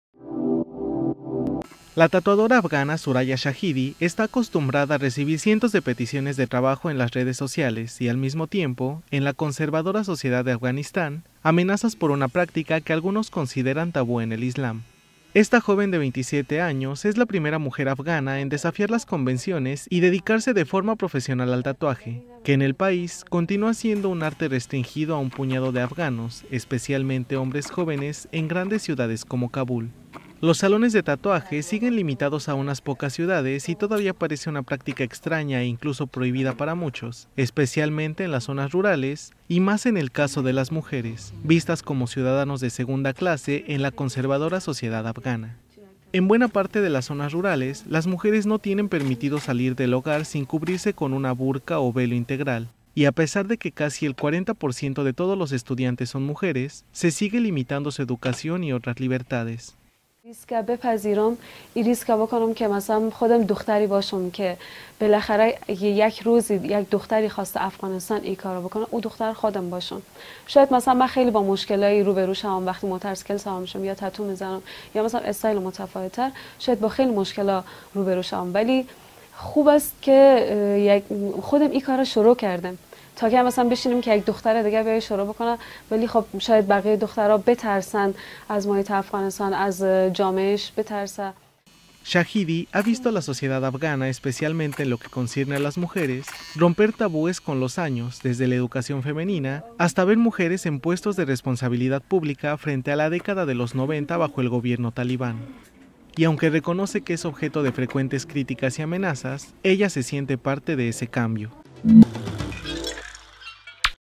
La tatuadora que rompe tabús y desafía a la conservadora sociedad afgana
La tatuadora afgana Suraya Shahidi está acostumbrada a recibir cientos de peticiones cada vez que comparte sus últimos trabajos en las redes sociales y, en la conservadora sociedad de Afganistán, también amenazas por una práctica que algunos consideran tabú en el islam.